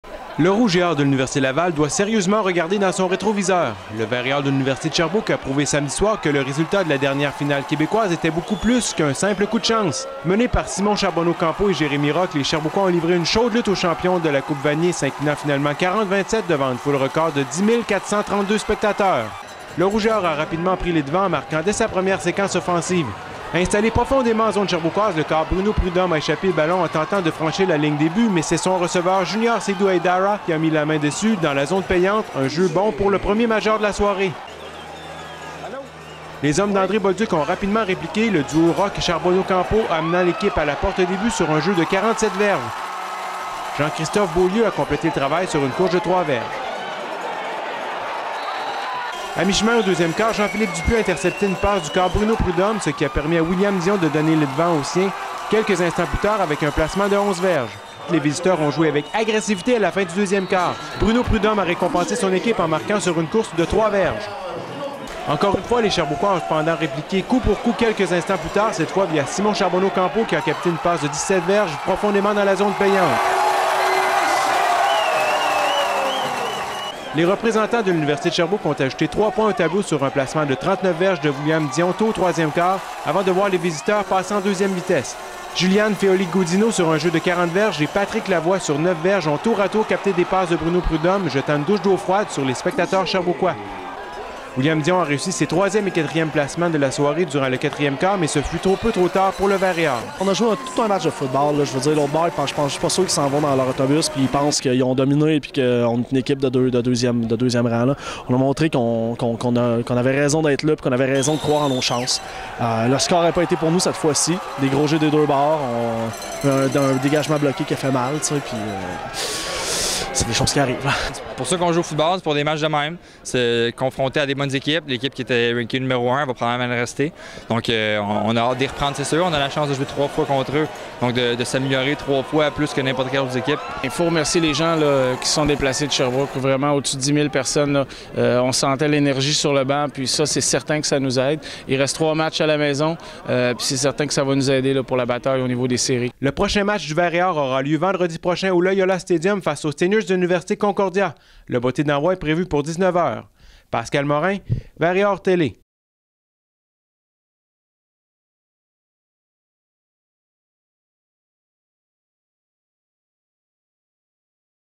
Reportage football - Vert & Or c. Laval_1_2011
Reportage sur le match opposant le Vert & Or au Rouge et Or de l'Université Laval, samedi le 17 septembre au stade de l'Université de Sherbrooke.